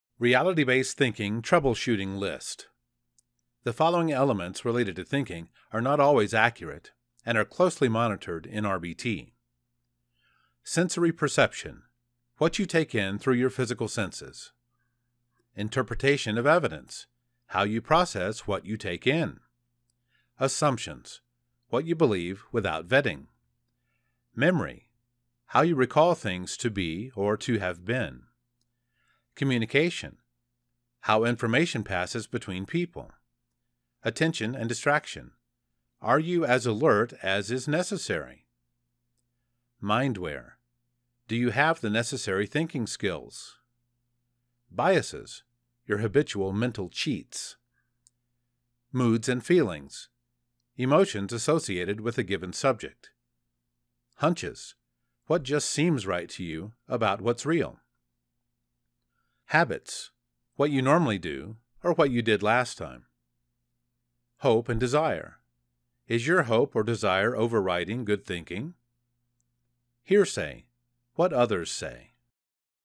RBT Troubleshooting List
Reality-Based Thinking Foundations
Sensory Perception.  What you take in through your physical senses.
Interpretation of Evidence.  How you process what you take in.
Assumptions.  What you believe without vetting.
Memory.  How you recall things to be, or to have been.
Communication.  How information passes between people.
Attention / Distraction.  Are you as alert as is necessary?
Mindware.  Do you have the necessary thinking skills?
Biases.  Your habitual mental cheats.
Moods / Feelings.  Emotions associated with a given subject.
Hunches.  What just seems right to you about what's real.
Habits.  What you normally do / what you did last time.
Hope / Desire.  Is your hope or desire overriding good thinking?
Hearsay.  What others say.